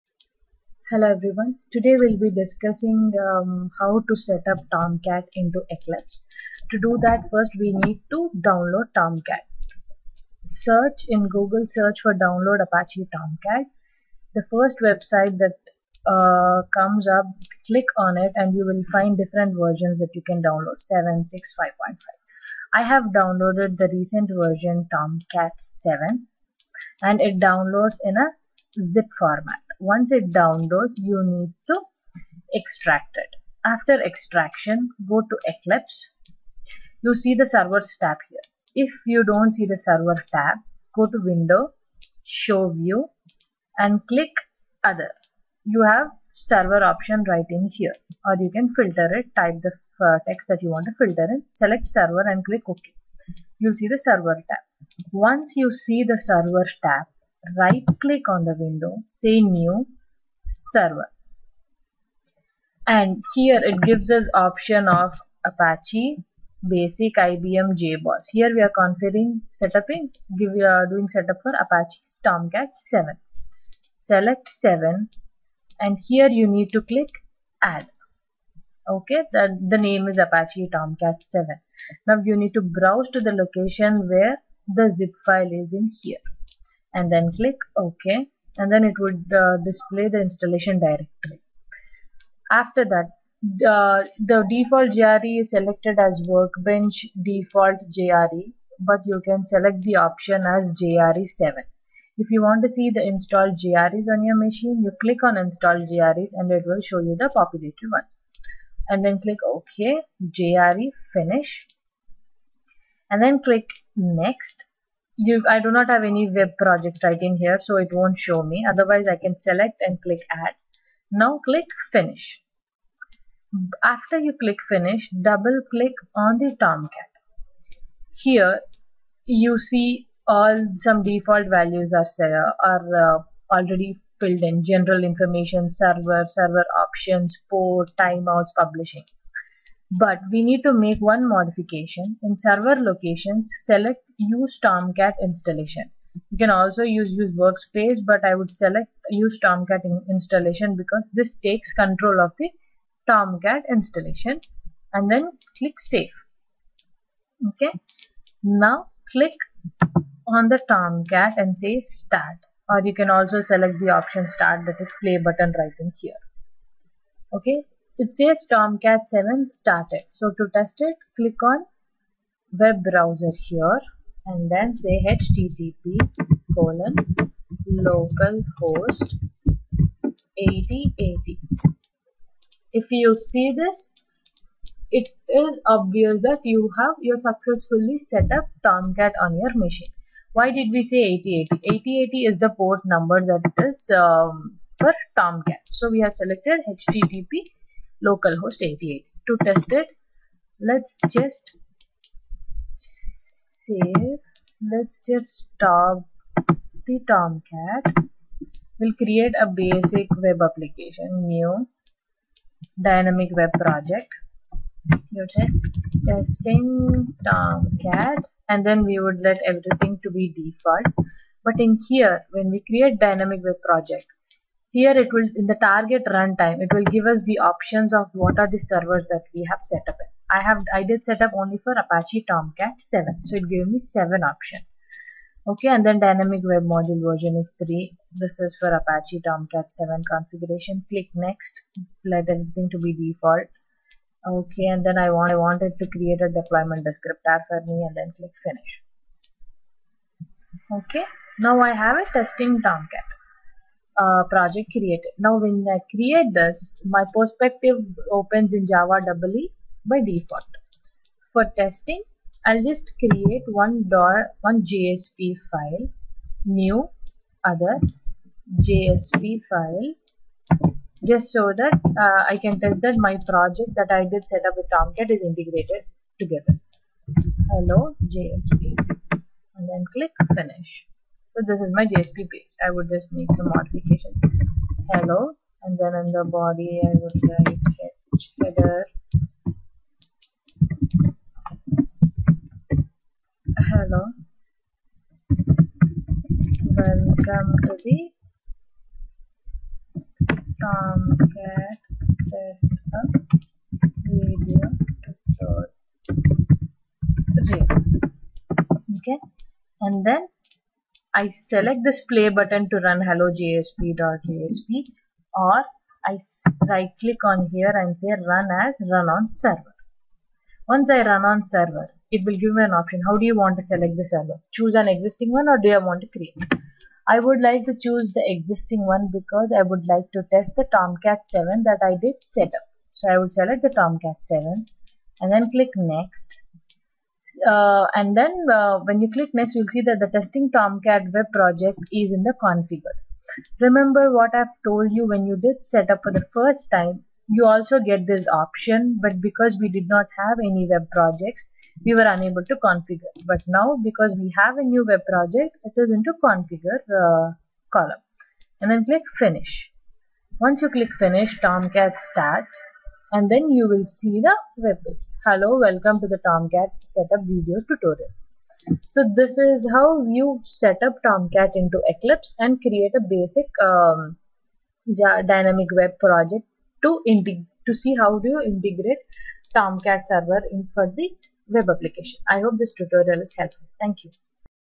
Tomcat setup in Eclipse
Setting up Apache Tomcat in Eclipse IDE  and testing by creating a web project.